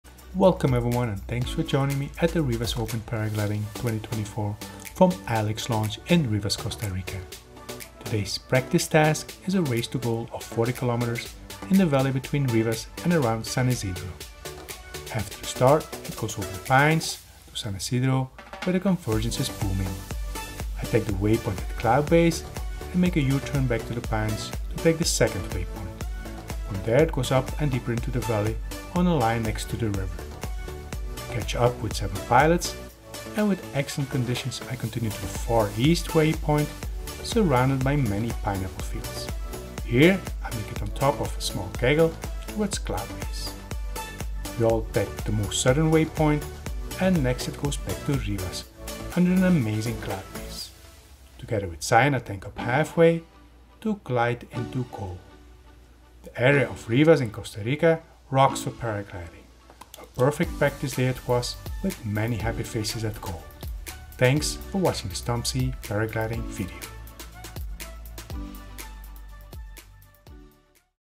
2024 Rivas Open Paragliding - Practice Task
The Rivas open takes place at Rivas in Costa Rica from Alex Launch.
Conditions are perfect for racing with lot's of thermal clouds in the wide valley in front of Launch.
Today was the first day of this 4 days event, and a practice Task was set